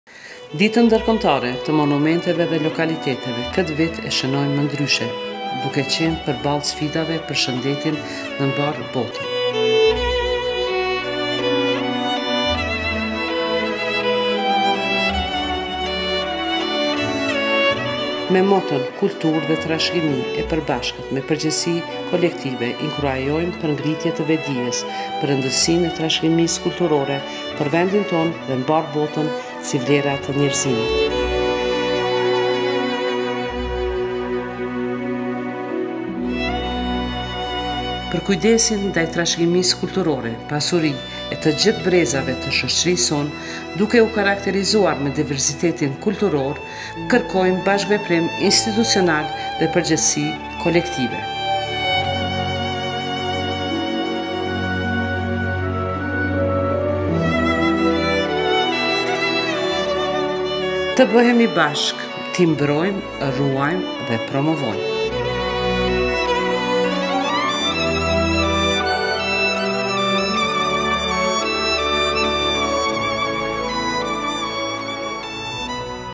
Adresimi i ministres Vlora Dumoshi me rastin e Ditës Ndërkombëtare të Monumenteve dhe Lokaliteteve
Ministrja Vlora Dumoshi, për shkak të rrethanave të pandemisë, ka përgatitur këtë adresim për t'iu drejtuar tërë komunitetit që merren me trashëgimi kulturore në Republikën e Kosovës